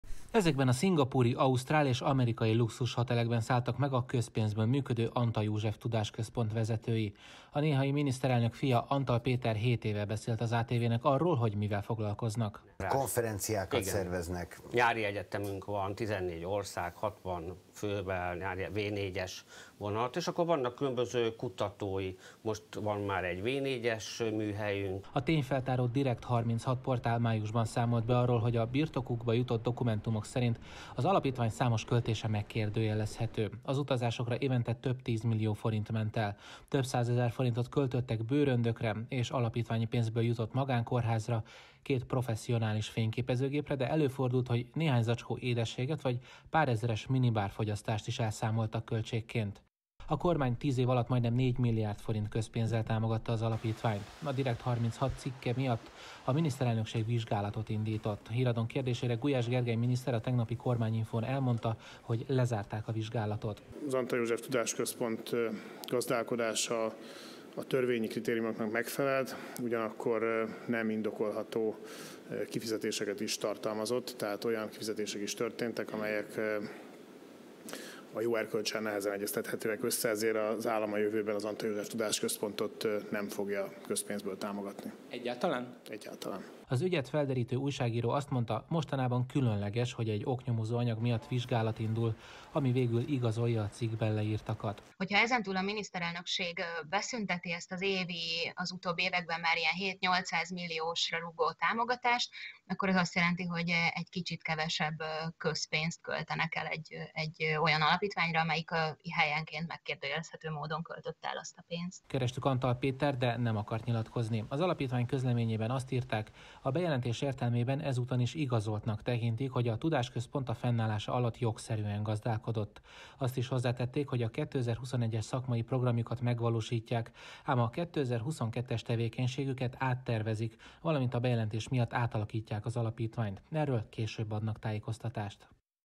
Nem ad több támogatást az állam az Antall József Tudásközpontnak 21-09-30
Nem ad több támogatást az állam az Antall József Tudásközpontnak, miután a Miniszterelnökség megvizsgálta a közpénzköltésüket, ezt az RTL Híradó kérdésére mondta el Gulyás Gergely miniszter a tegnapi kormányinfón. A Direkt36 tényfeltáró portál még májusban írta meg, hogy luxusutazásokra például több tízmillió forintot költött évente a néhai miniszterelnök fia által vezetett alapítvány. Az Antall József Tudásközpont azt írta, a vizsgálat szerintük igazolta, hogy jogszerűen gazdálkodtak.